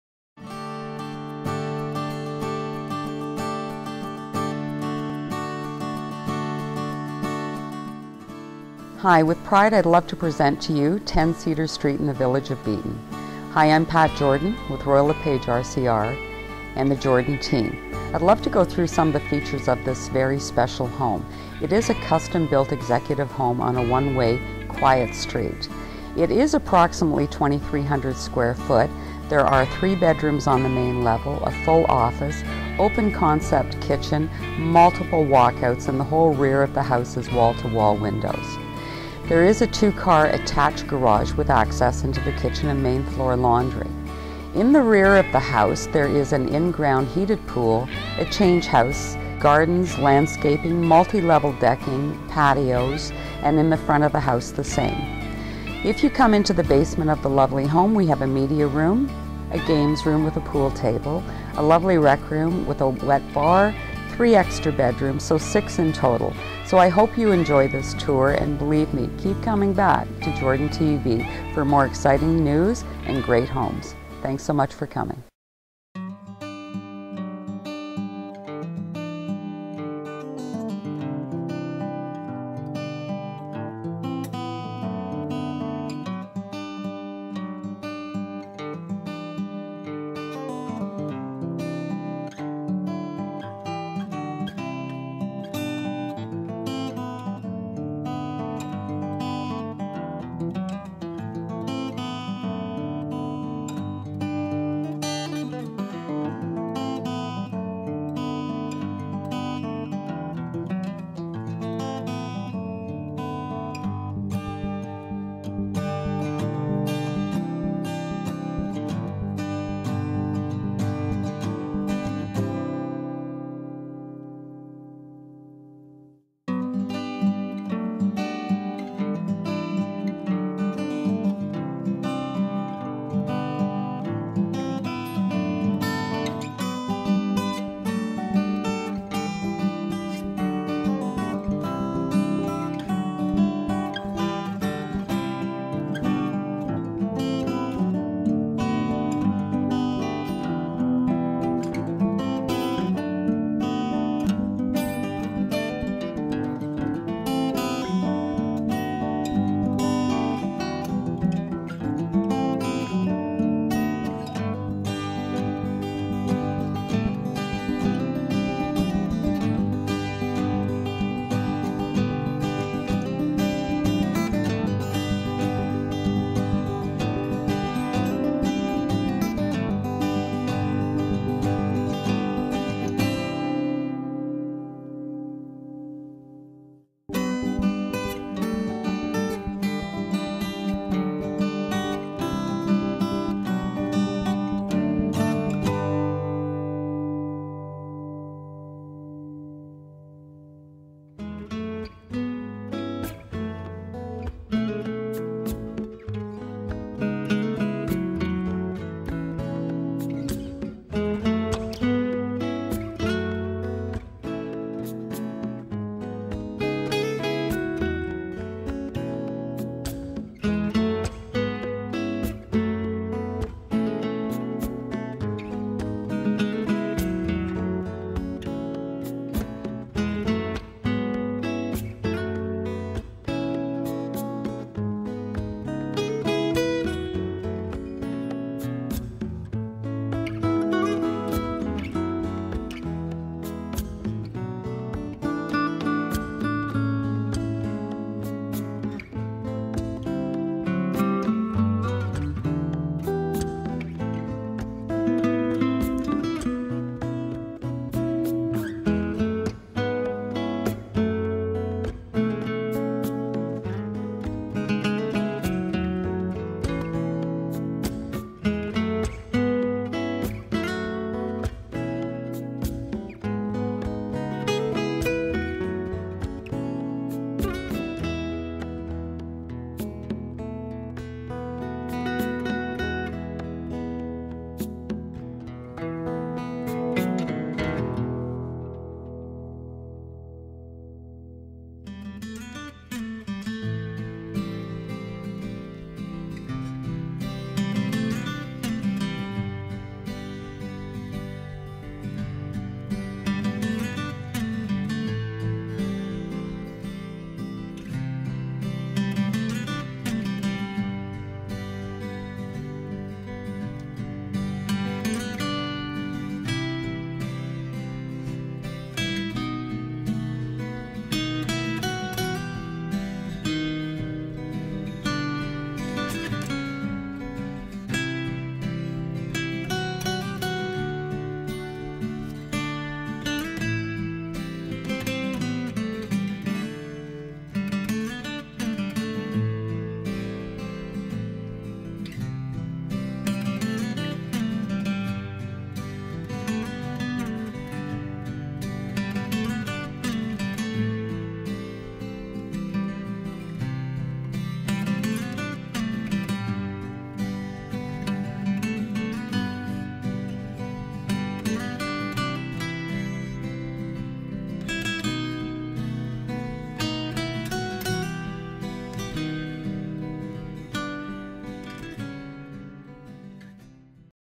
10 Cedar St Beeton Ontario
One Of A Kind Custom Built Home First Time On The Market! Ahouse Like This Won't Last!!
Pool House Has Hydro, Whole House Has Crown Moulding, Basement Has Rec/Games/ Media Rooms With 3 Pc And Gas Frpl, Storage Room And 3 Full Bedrooms. Home Was Built In 1990.